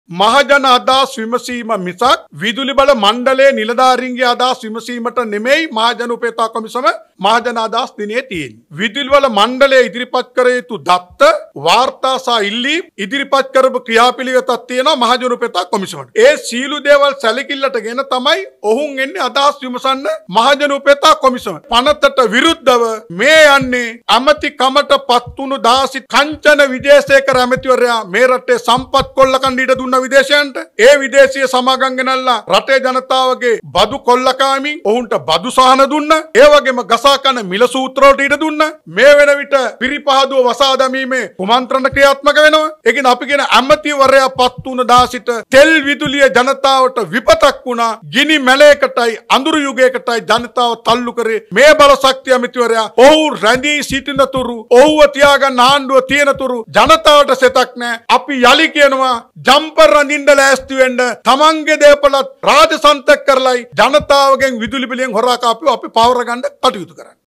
ආනන්ද පාලිත මහතා කියූ කංචනගේ කෙරුවාව 2024.02.17
ආනන්ද පාලිත මහතා මාධ්‍ය හමුවේ දැක්වූ අදහස්...